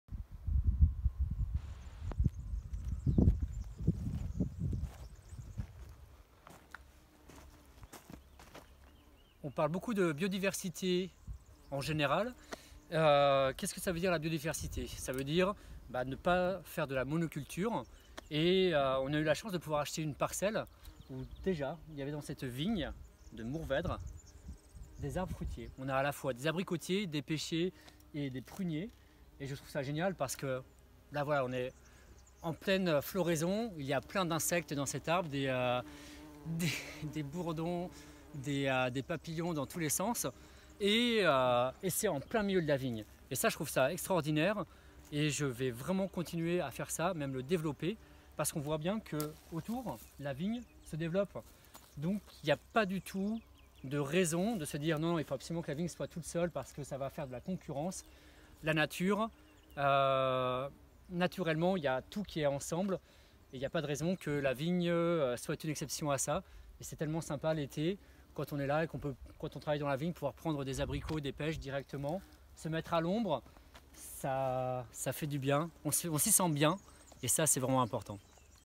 L'importance de la biodiversité au cœur des vignes : un témoignage de Vincent Chansault
Vincent Chansault, vigneron au Domaine Gayda nous explique l'importance d'une biodiversité au cœur des vignes. Le développement d’arbres fruitiers est complémentaire avec celui de la vigne (ne pas faire de mono-culture).